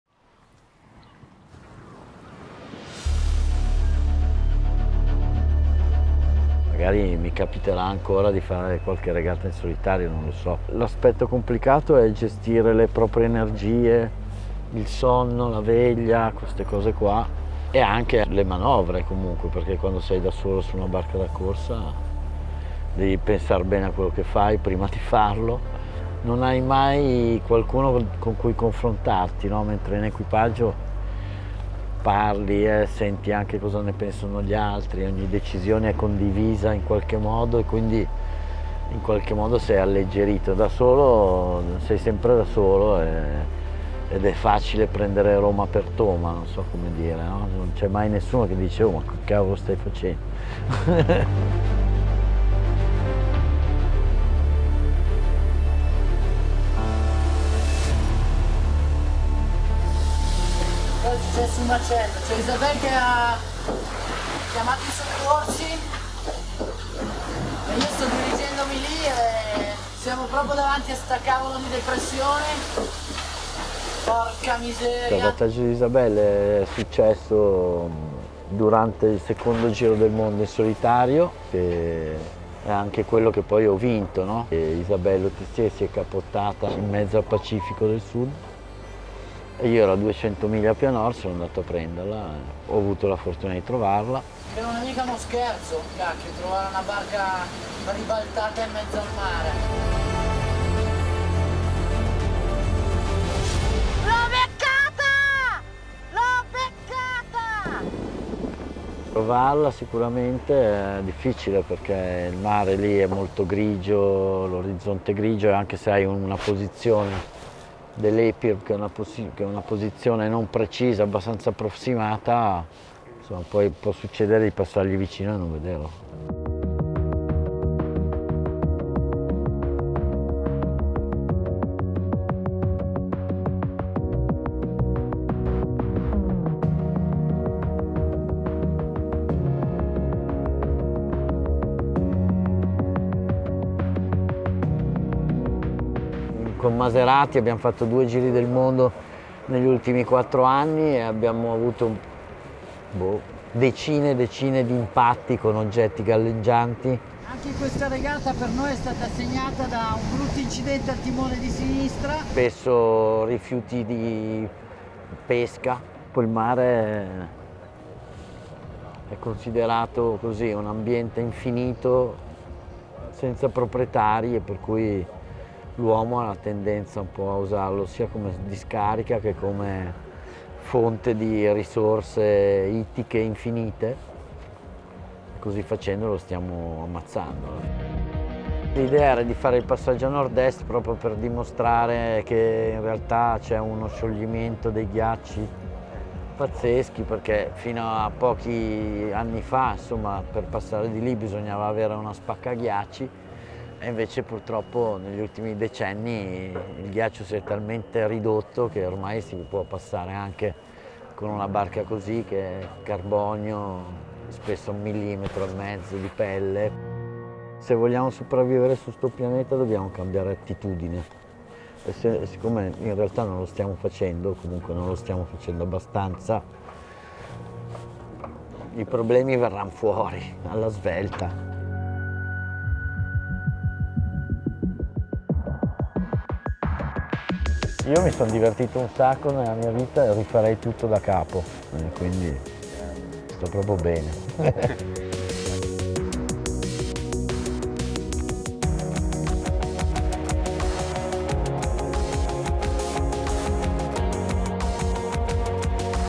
Giovanni Soldini - Garmin Beat Yesterday
Giovanni Soldini non ci ha pensato due volte a cambiare rotta e buttarsi nella tempesta per salvare Isabelle Autissier, cappottata nel Pacifico del Sud durante il Giro del Mondo in solitario del 1999. Ma a distanza di oltre 20 anni il suo Beat Yesterday non riguarda più competizioni e regate: la sostenibilità e la salute dei mari deve diventare una priorità per tutti.

“Consideriamo il mare come una discarica e una fonte di risorse ittiche infinita, ma così lo stiamo ammazzando. Se vogliamo sopravvivere dobbiamo cambiare”.

Raccontaci la tua impresa e candidati per vincere il Garmin Beat Yesterday Awards